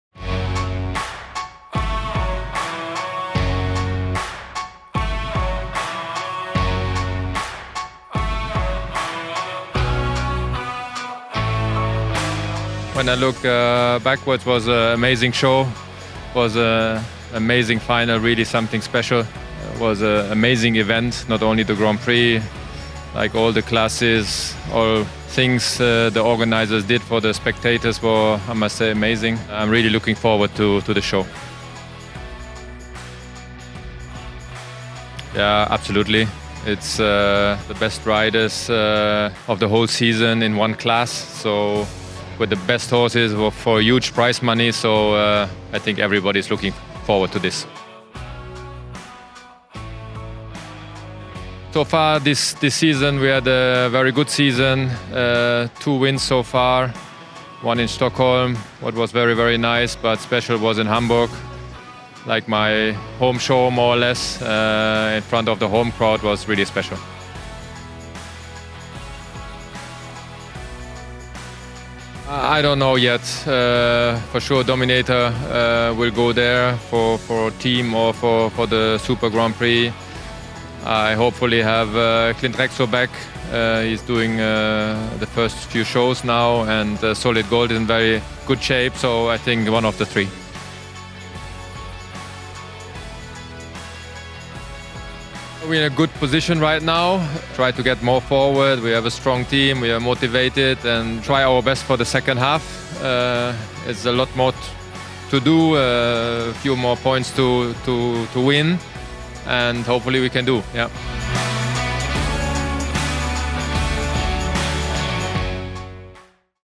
Do Prahy určitě přivezu Dominatora, uvedl lídr LGCT Christian Ahlmann
Němec Christian Ahlmann je momentálně lídr žebříčku světové parkurové série Longines Global Champions Tour. Letos si při ní připsal již dvě vítězství, a tak bude pochopitelně i jedním z favoritů pro Super Grand Prix v Praze.

Před čtrnáctým kolem Longines Global Champions Tour v New Yorku, které se pojede v sobotu 24. září, je Christian Ahlmann lídrem tabulky s 238 body. Do Spojených států amerických se však nevypravil, a tak se může stát, že jej o průběžné prvenství připraví už za pár dní Brit Ben Maher, jenž na něj ztrácí pouhých šest bodů.

Vše se do konce sezony ovšem může ještě změnit při posledním mítinku v Rijádu. Ten se uskuteční v druhé polovině října, tedy necelý měsíc před galashow světového parkuru Global Champions Prague Playoffs v libeňské O2 areně, kde nepochybně Christian Ahlmann chybět nebude. Po vítězství při šestém kole v Hamburku je totiž jedním z držitelů zlaté vstupenky do sobotní Super Grand Prix.

Aktuálně dvacátý jezdec světového žebříčku Mezinárodní jezdecké federace (FEI Longines Rankings) se do hlavní soutěže jednotlivců LGCT kvalifikoval již podruhé. Povedlo se mu to i v roce 2019. „Vzpomínám na Prague Playoffs jako na výborné závody. Bylo to skvělé finále, něco jedinečného. Je to skvělá akce a nemyslím tím jen Super Grand Prix, ale všechny úrovně soutěží. Bylo úžasné vidět, co všechno pořadatelé připravili pro diváky. Velmi se na letošní ročník těším,“ uvedl v rozhovoru pro štáb organizačního týmu Prague Playoffs.

„Účast v letošní Super Grand Prix je pro mě opravdu důležitá. Je to soutěž, ve které startují ti nejlepší jezdci celé sezony s jejich nejlepšími koňmi. Bojuje se navíc o obrovskou výhru. Myslím, že se na to všichni těší,“ připomíná Ahlmann skutečnost, že v této soutěži se bude soutěžit o astronomickou dotaci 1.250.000 eur, přičemž vítěz si přijde na rovných 300.000 eur, druhý umístěný bere 200.000 a třetí o dalších 50.000 méně. Výhru má však zajištěnu i poslední patnáctý účastník.

Zmíněný německý jezdec určitě pomýšlí na příčky nejvyšší, ostatně stále má reálnou šanci stát se šampionem základní části seriálu. Letos vyhrál už dvakrát. Kromě Hamburku se mu to povedlo i při osmém kole ve švédském Stockholmu. Místo něj tak bral zlatou vstupenku druhý umístěný Henrick von Eckermann, jemuž se podařilo vyhrát Super Grand Prix v loňském roce. „Letos mám velmi dobrou sezonu. Již jsem vyhrál dvakrát. Stockholm byl velmi krásný, ale nejkrásnější vzpomínky mám na Hamburk. Byly to v podstatě mé domovské závody a mít možnost startovat před domácím publikem bylo pro mě něco speciálního,“ popisuje Ahlmann.

Které z koní si vezme do nejtěžší soutěže sezony v pražské O2 areně, to ještě s jistotou neví. „Dominator 2000 Z stoprocentně pojede ať už za tým, nebo v Super Grand Prix. Doufám, že budu s sebou mít zpět hřebce Clintrexo Z. Absolvoval první dvoje závody. Solid Gold je také ve skvělé formě, takže bych řekl, že jedničkou bude jeden z těchto tří,“ nastínil svůj plán. V týmové Global Champions League je Christian Ahlmann členem celku Shanghai Swans, jenž je s 217 body třetí před čtvrtými Prague Lions. O celkové prvenství se pravděpodobně poperou Berlin Eagles a Stockholm Hearts, kteří mají náskok více jak čtyřiceti bodů, ale Shanghai Swans si věří pro GCL Super Cup, jenž je také součástí Prague Playoffs. „Prozatím máme dobrou pozici. Máme silný tým a motivaci. Snad se nám v Super Cupu podaří uspět,“ doufá jeden z nejúspěšnějších jezdců letošní Global Champions.

GC Prague Playoffs se v pražské O2 areně uskuteční od čtvrtka 17. do neděle 20. listopadu. Součástí akce bude i velká koňská show pro rodiny s dětmi a další příznivce jezdectví. Během všech čtyř dnů vystoupí „Létající Francouz“ Lorenzo. Ten se svými koňmi také skáče přes překážky, ale vypadají úplně jinak než ty parkurové a Lorenzo při tom nesedí v sedle, ale stojí přímo na hřbetech svých koňských partnerů. Podrobnosti o programu najdete na internetových stránkách

Foto: Josef Malinovský